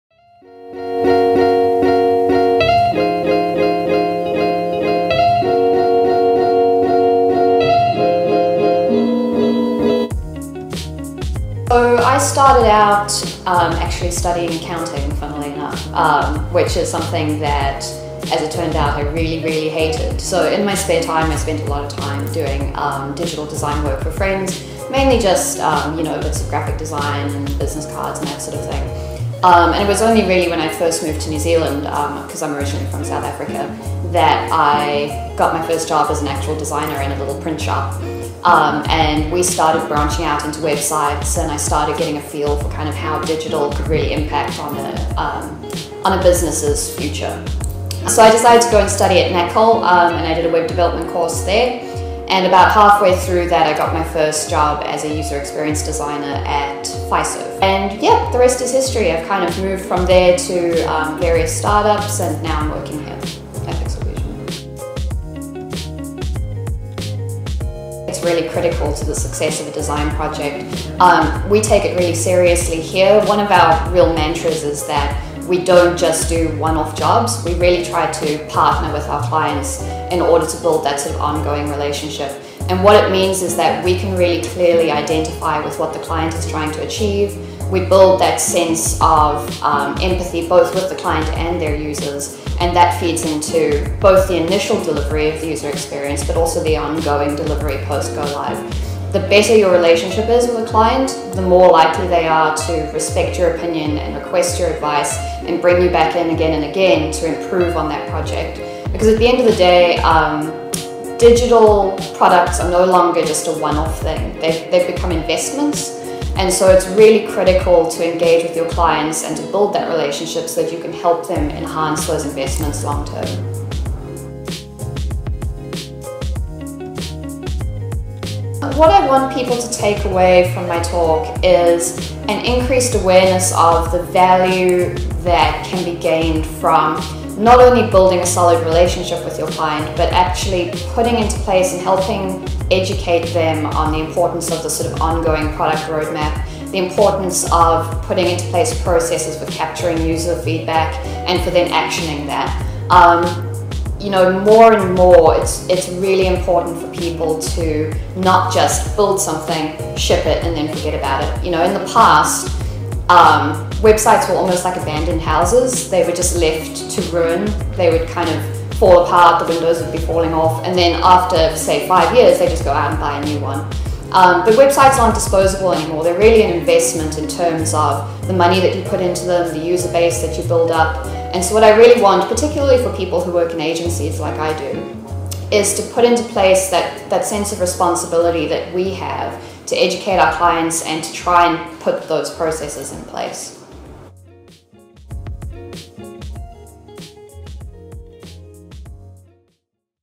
UX Homegrown Katherine Barrow Interview
Katherine Barrow chats about her path into UX and her upcoming talk for UX Homegrown